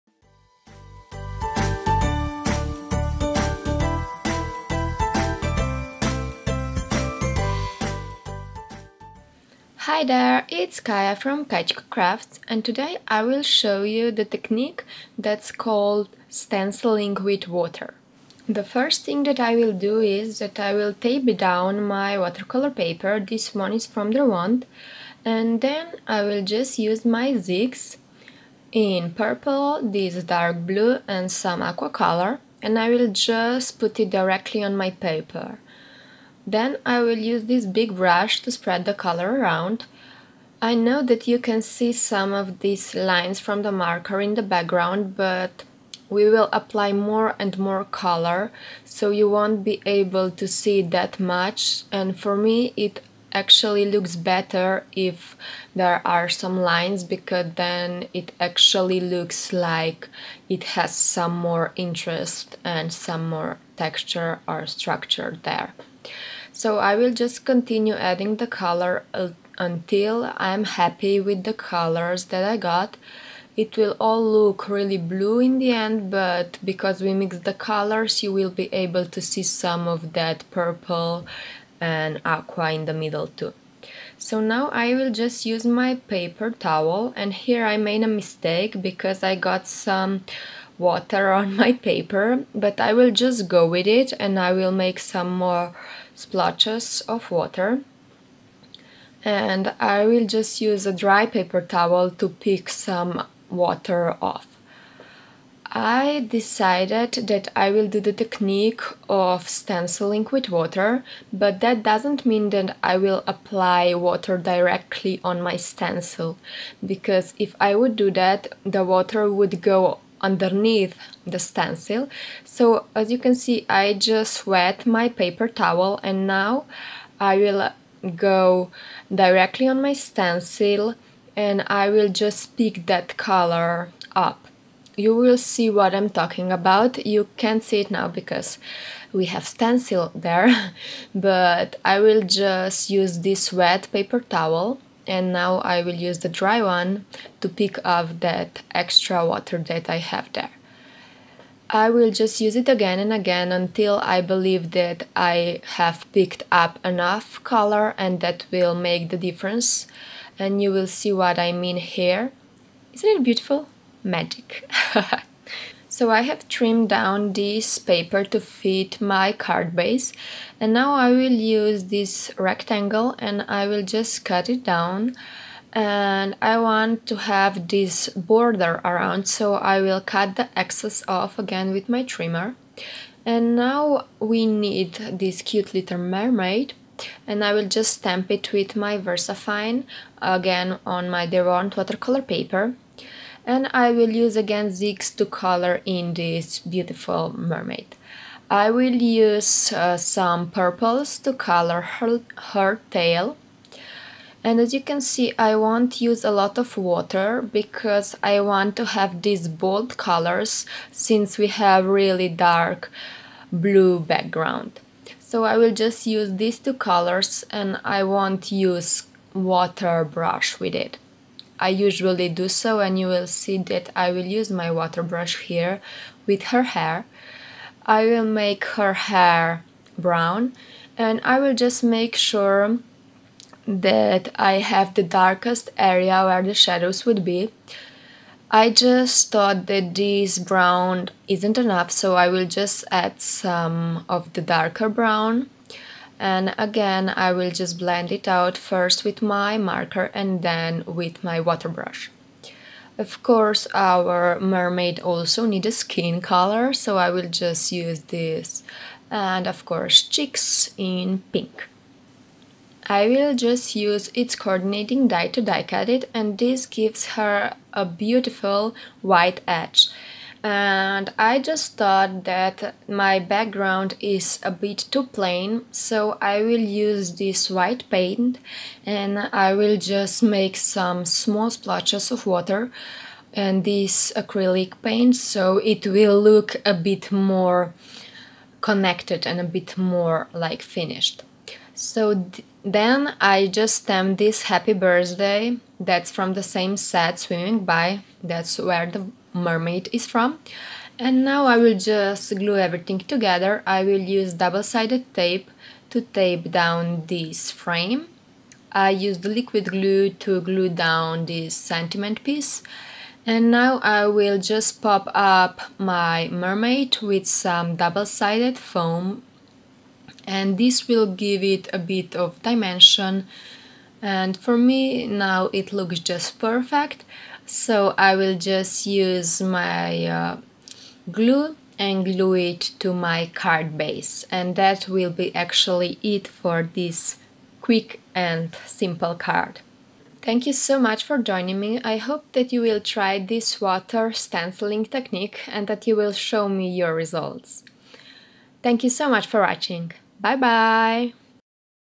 How to use stencil with water
Join me as I show you how to use stencil with water!

• Versafine - ONYX BLACK
• Kuretake - water brush
• Derwent - smooth watercolor paper
• Zig clean color - 48 PACK